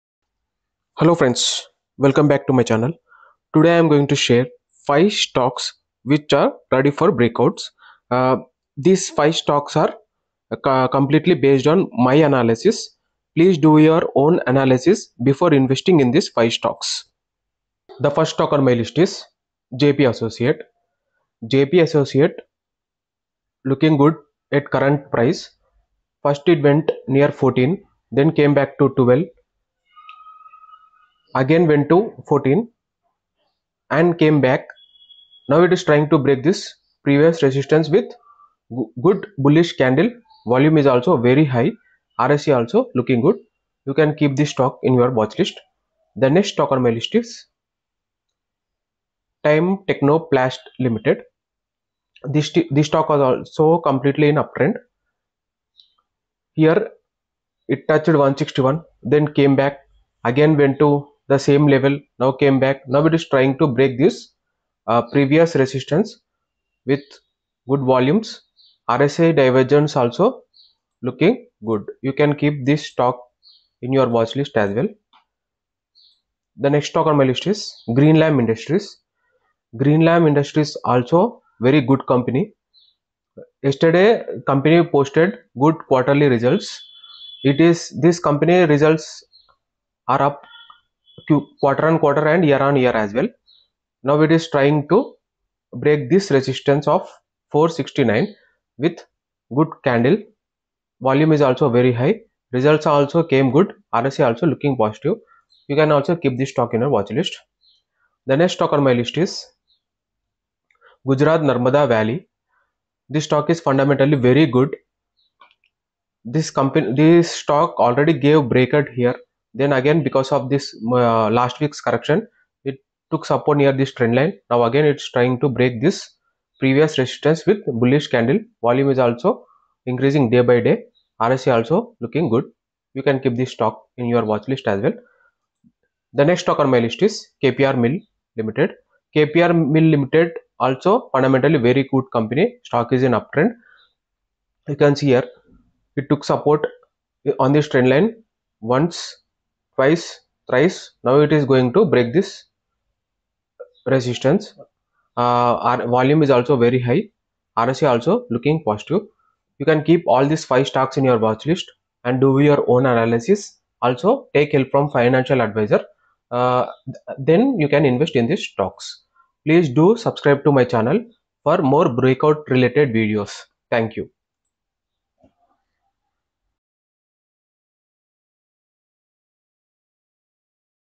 5 Breakout Stocks For Tomorrow | Swing Trading Stocks
📈 Key Highlights:

In-depth analysis of each top stock pick.
Why these stocks are considered the best for 2023.
Risk assessment and potential rewards.
Expert insights on market conditions and strategies.
Whether you're an experienced investor or new to the world of stocks, our recommendations are designed to help you make informed investment decisions.